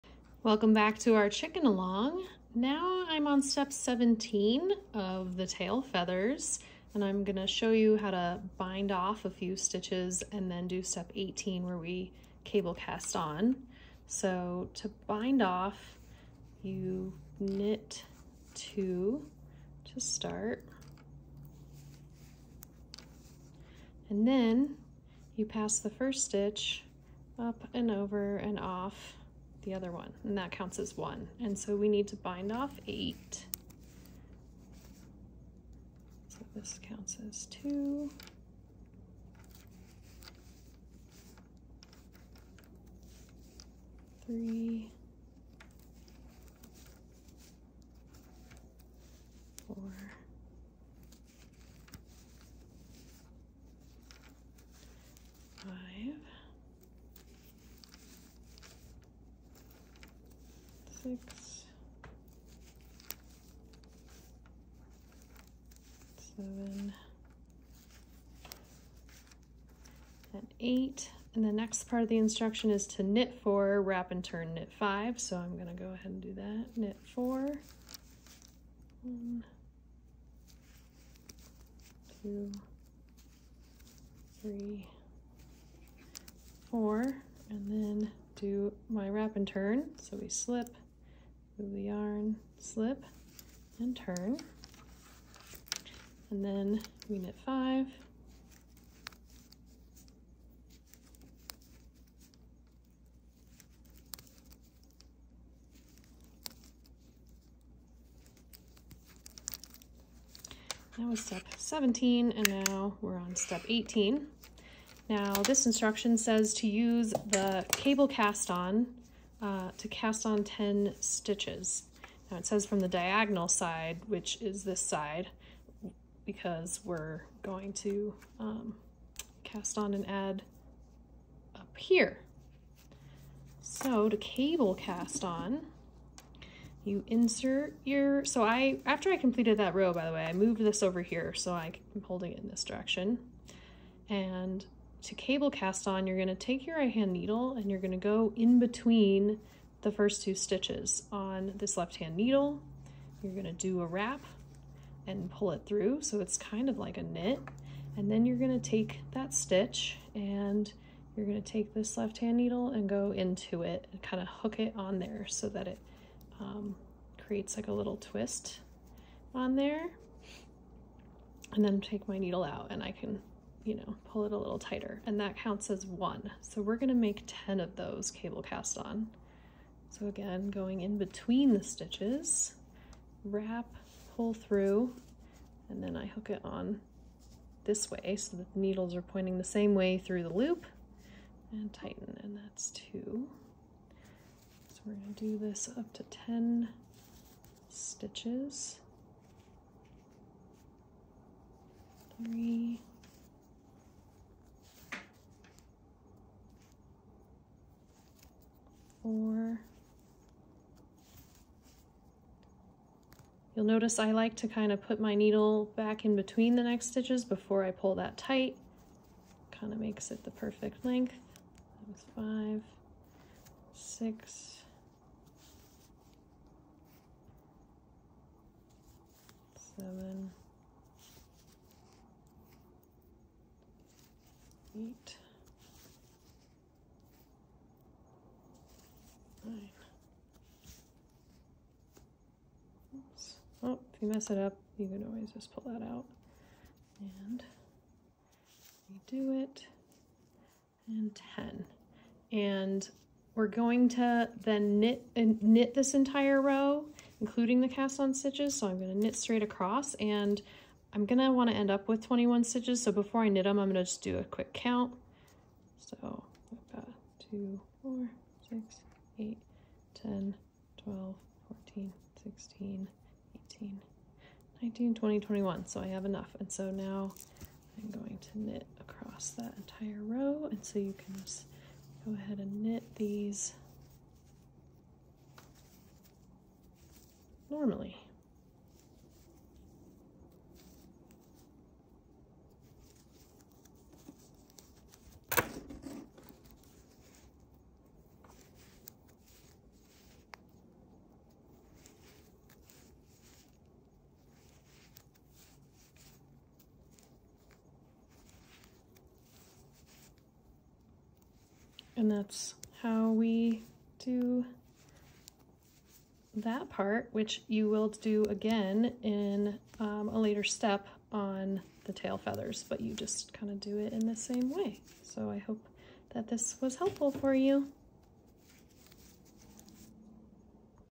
Emotional Support Chicken Bind off and Cable Cast On sts for Tail Feathers
In this video, I show you how to bind off sts, perform a wrap and turn (in garter st) and then cable cast on more stitches to create the tail feather shaping of the Emotional Support Chicken pattern.

Visit astraknots.com for more knitting videos, sign up for online knitting classes, or get knitting help!